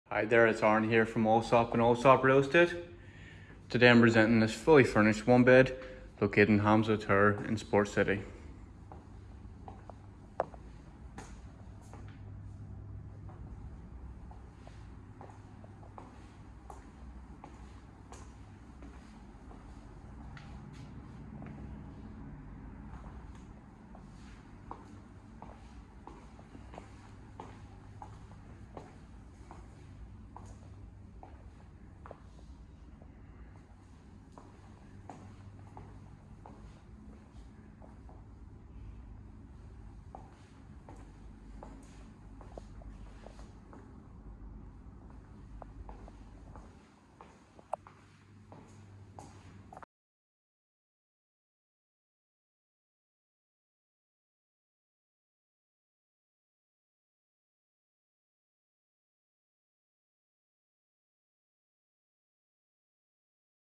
1 Bed Apartment in Hamza Tower, Dubai Sports City
This Spacious 1 Bedroom Apartment Is Available To Rent Now

- 904 Sq. Ft.
- Large Balcony
- Shared Pool
- Covered Parking
- 24/7 Security
- Fitness Center
- Communal Gardens
- Large Living Space

This Modern 1 Bed Apartment with partial Golf Course views is in Hamza Tower which is located in Sports City. As residents you can enjoy and experience world class sports facilities such as the four fantastic stadiums. Sports City provides a mix of residential, retail, leisure and recreational facilities.